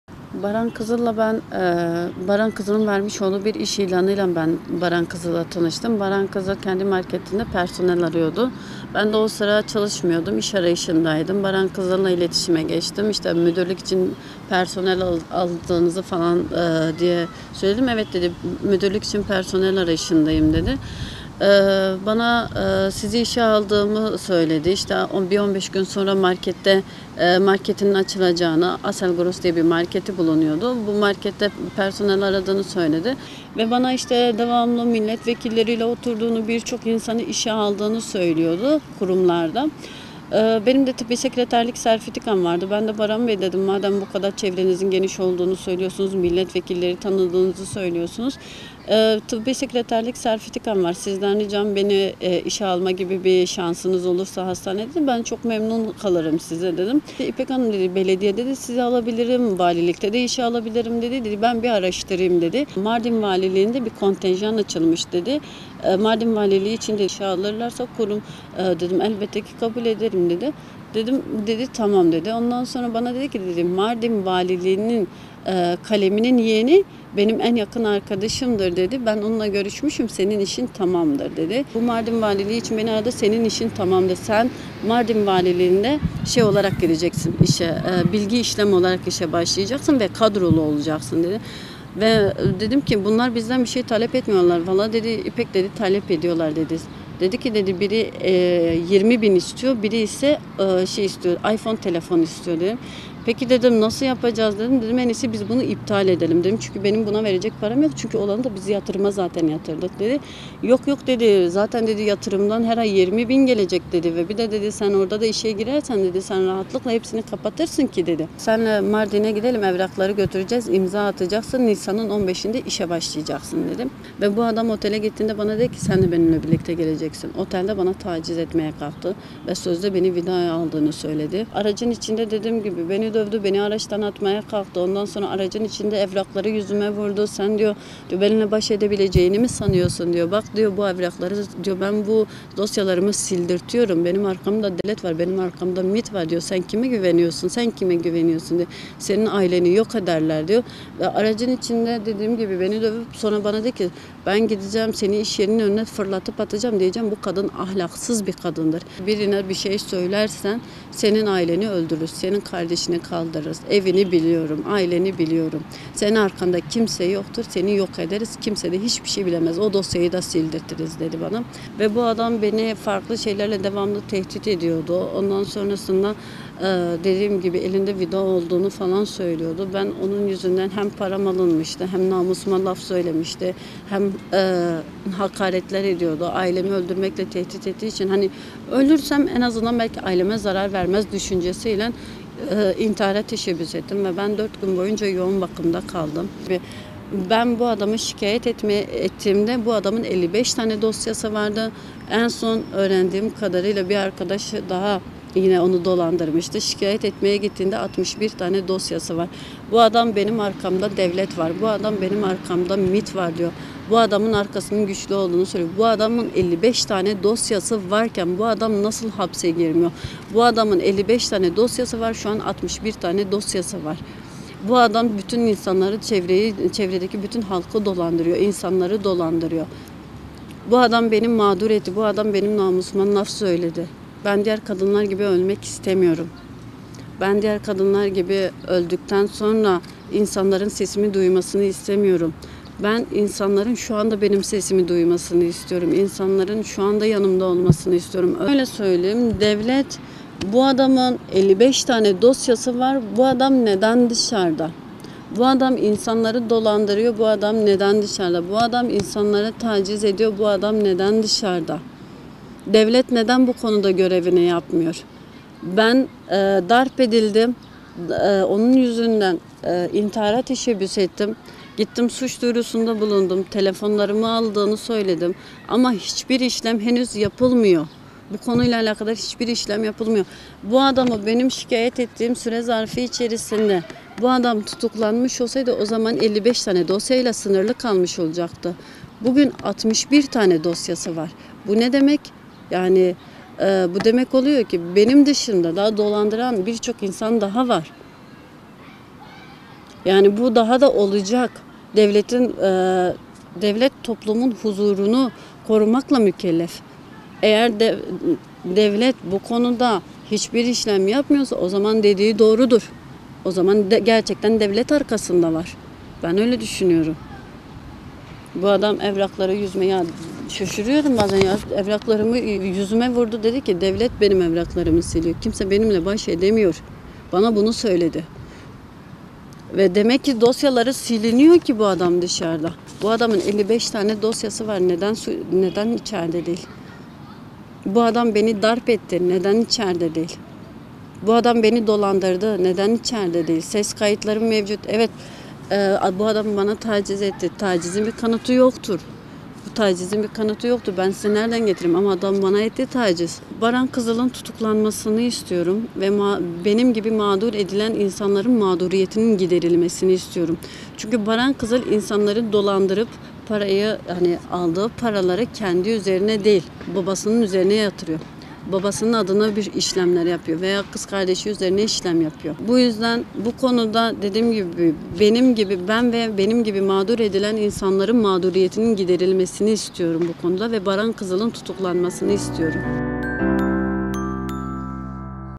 'ARKAMDA DEVLET VAR' DİYEREK TECAVÜZ GİRİŞİMİNDE BULUNDU
Diyarbakır’da yaşayan İpek Gülşen, iş başvurusu üzerine Ocak ayında tanıştığı Baran Kızıl adlı erkek tarafından önce dolandırıldı, taciz edildi ve tecavüz tehditlerine maruz kaldı. Gülşen, ölüm tehditleriyle devam eden süreci Mezopotamya Ajansı’na (MA) anlatarak, kadınlara destek ve dayanışma çağrısı yaptı.